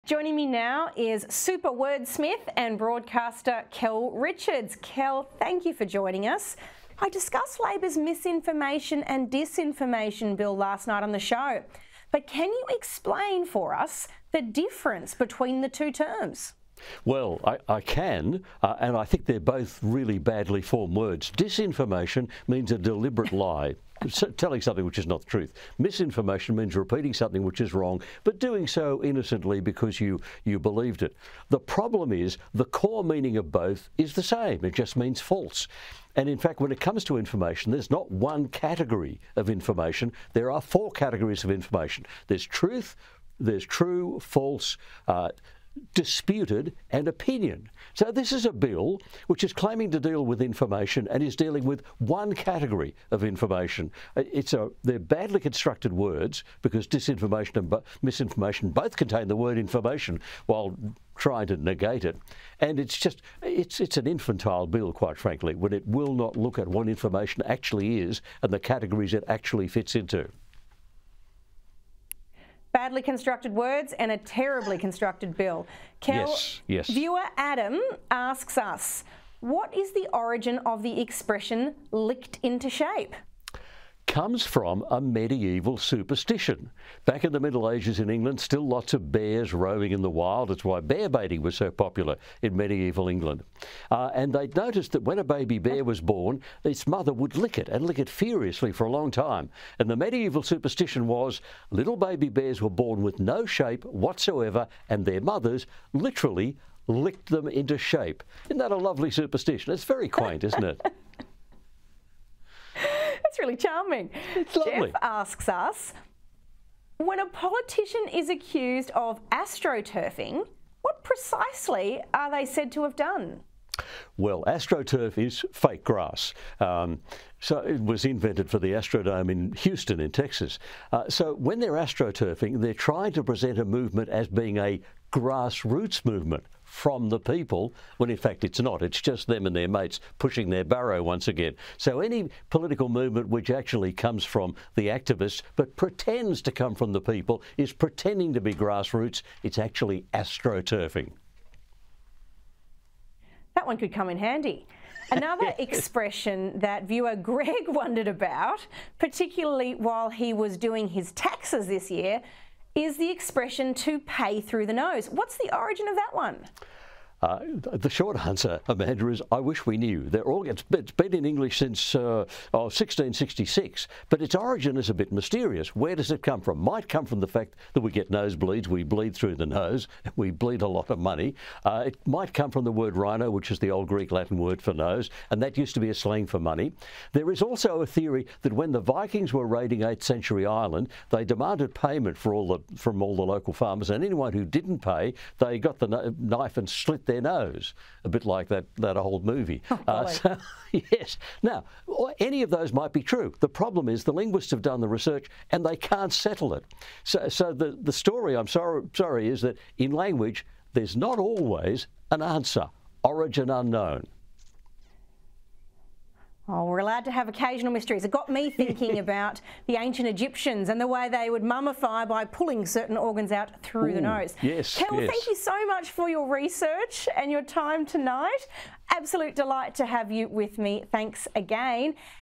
Words Matter with Kel Richards: The meanings behind 'misinformation' and 'disinformation'
Wordsmith and broadcaster Kel Richards says misinformation and disinformation are “badly constructed words” as they contain the word ‘information’ while trying to negate it.